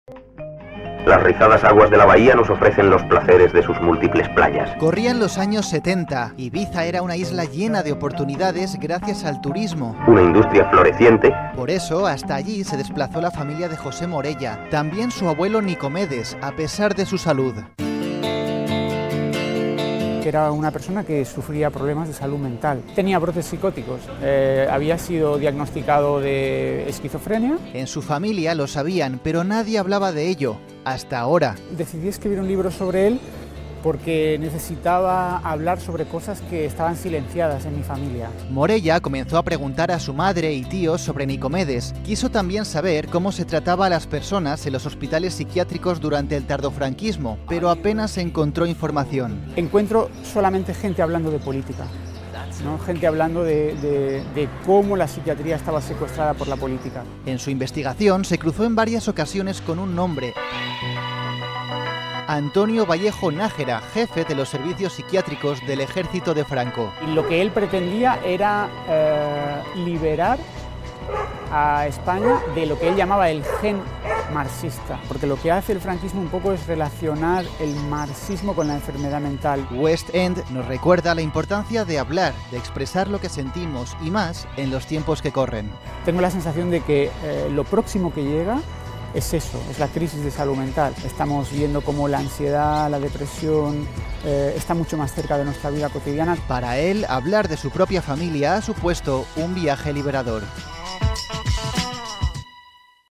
José Morella: "Creo que lo próximo que llega es la crisis de salud mental" | Ahora qué leo
En 'West End' (Siruela, 2019), José Morella mezcla testimonios familiares, reflexiones e investigaciones para desenterrar una historia familiar silenciada durante años: la de su abuelo Nicomedes Miranda, un hombre con problemas de salud mental.